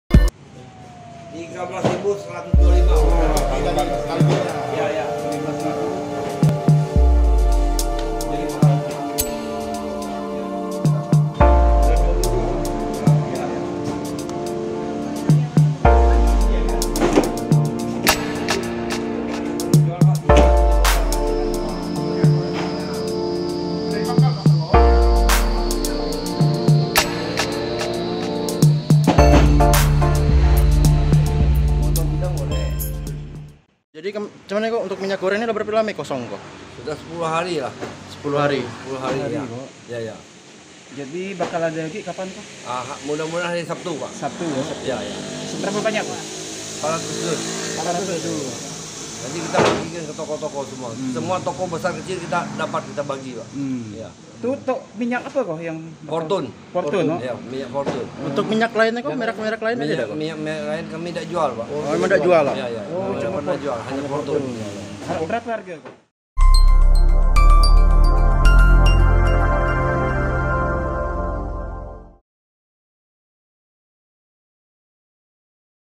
Stok Minyak Goreng Di Gudang Masih Kosong, DKUP Bangka Barat Sebut Akan Ada Distribusi 400 Dus
BANGKAPOS.COM,BANGKA- Ditengah terjadinya kelangkaan minyak goreng khususnya yang di subsidi oleh pemerintah, sebanyak 400 dus minyak goreng merek Fortune akan disuplai ke Kabupaten Bangka Barat dalam waktu dekat.

Hal ini pun dibeberkan Kepala Dinas Koperasi, UMKM, dan Perindustrian Kabupaten Bangka Barat, Aidi terkait rencana suplai minyak goreng yang akan tiba esok hari.

"Kemarin masuk 100 dus ke Muntok saja, tapi hari ini kondisinya mulai menipis lagi. Kita sudah koordinasi dengan agen, karena besok pagi diperkirakan akan ada 400 dus untuk Bangka Barat," ujar Aidi, Kamis (17/02/2022).

(Bangkapos.com/Rizky Irianda Pahlevy).